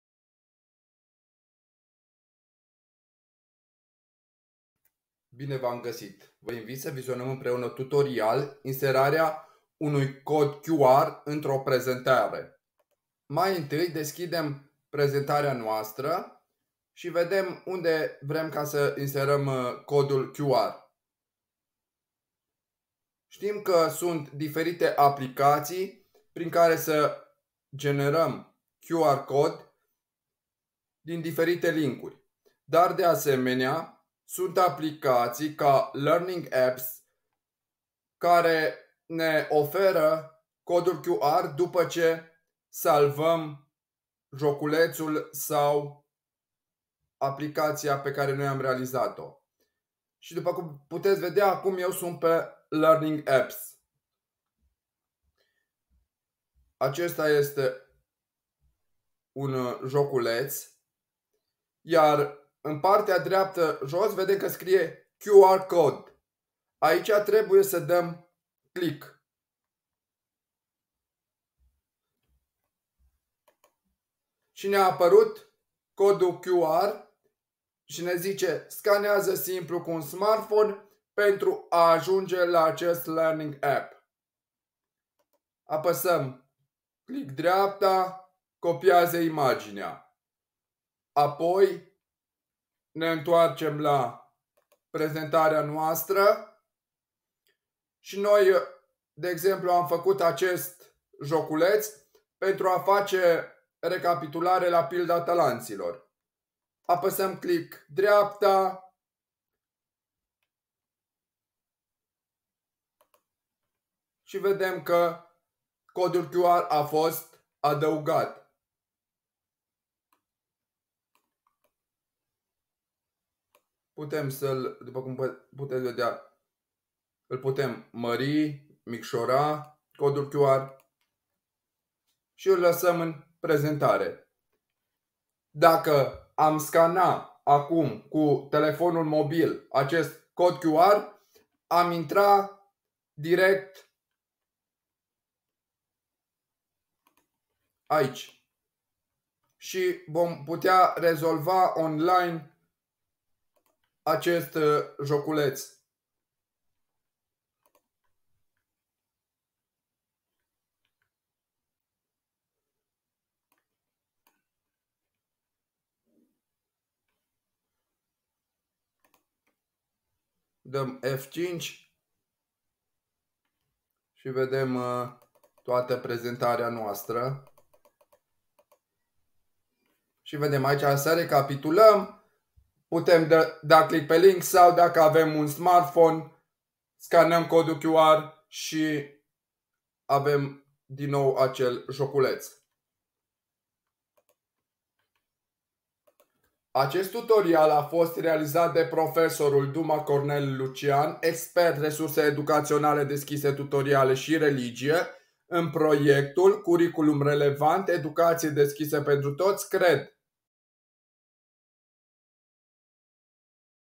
Tutorial cum inserăm un cod QR într-o prezentare
Această resursă educațională deschisă de tip video este un tutorial/ghid care vine în sprijinul profesorilor sau elevilor care doresc să integreze un cod QR de pe Learningapps într-o prezentare. Am prezentat de unde se copie un cod QR de la un exercițiu realizat în aplicația LearningApps pentru a putea fi postat apoi într-o prezentare Power Point.
În acest tutorial am prezentat cum se poate insera un cod QR într-o prezentare simpu și rapid.